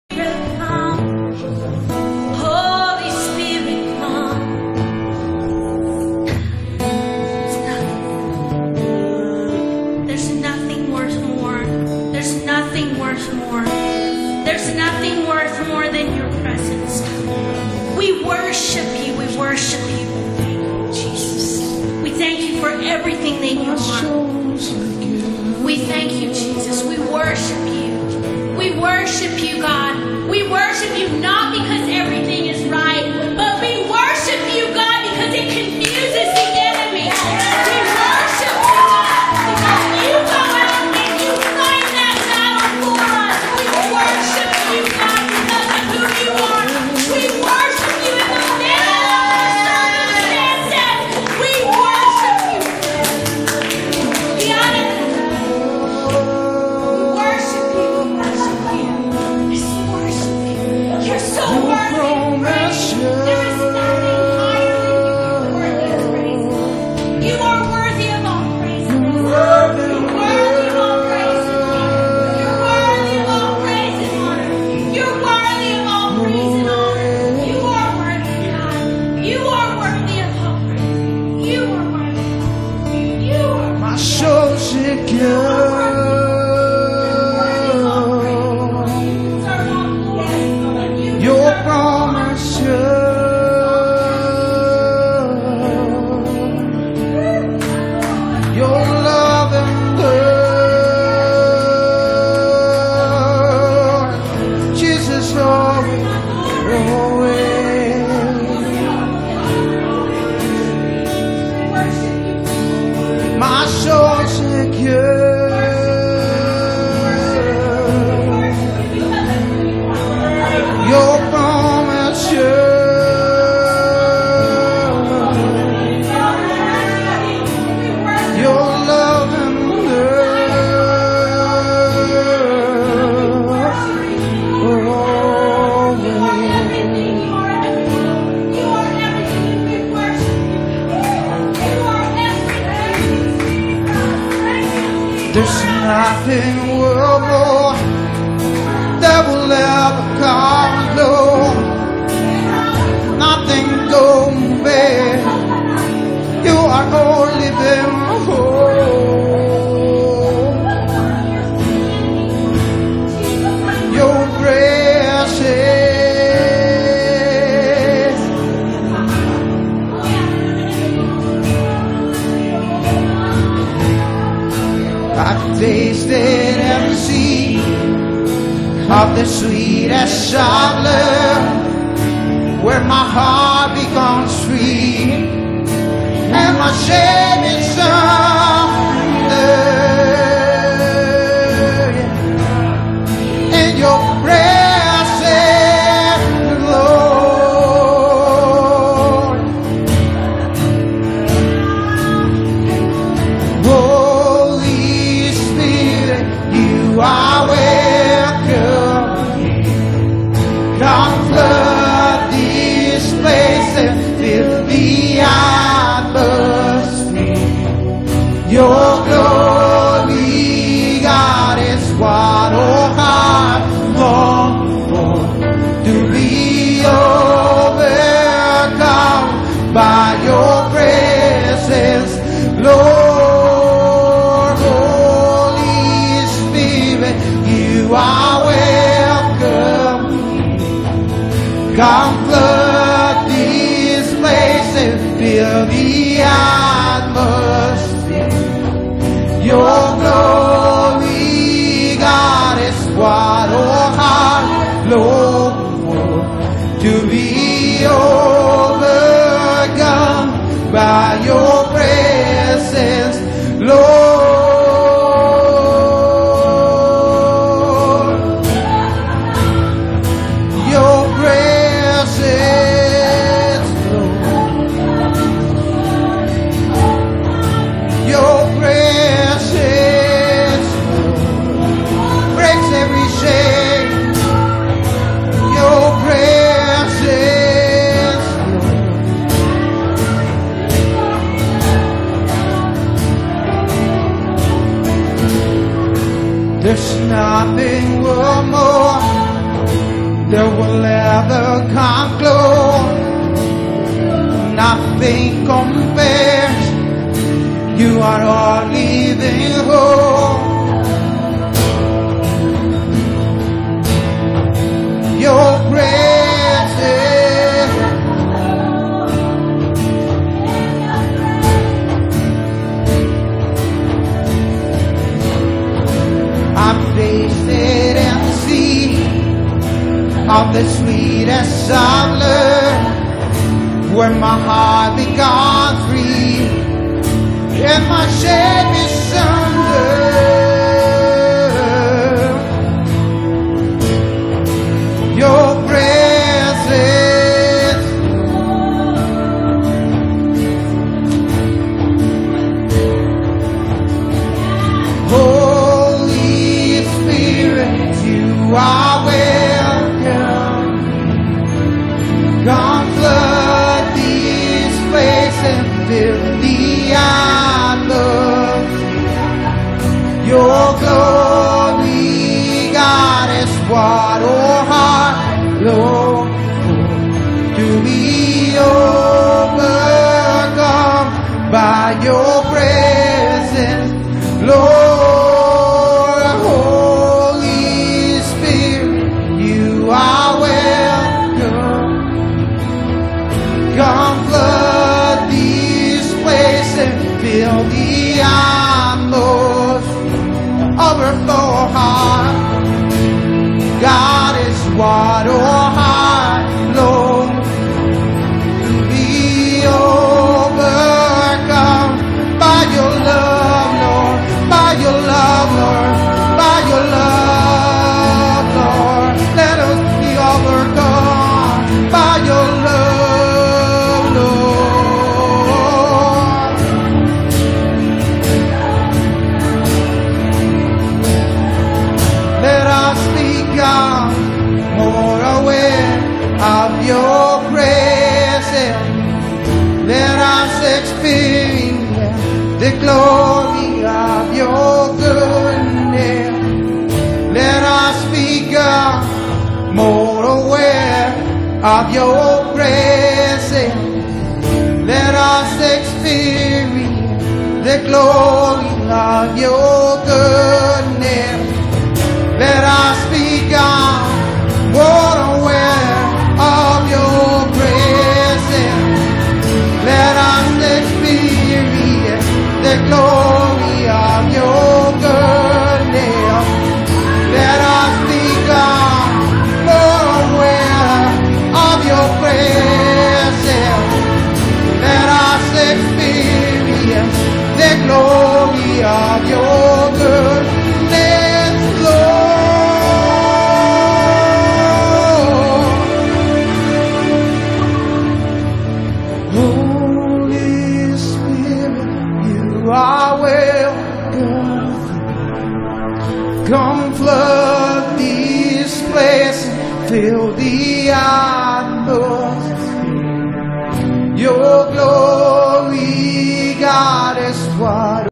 Faith for Life Church UT SHOW ME YOUR GLORY PT3
FAITH FOR LIFE CHURCH IN UTAH , SUNDAY 7-6-14
SOMETIMES WE JUST NEED TO BE IN HIS PRESENCE, AND WORSHIP THE CREATOR OF ALL THINGS ,ALMIGHTY GOD !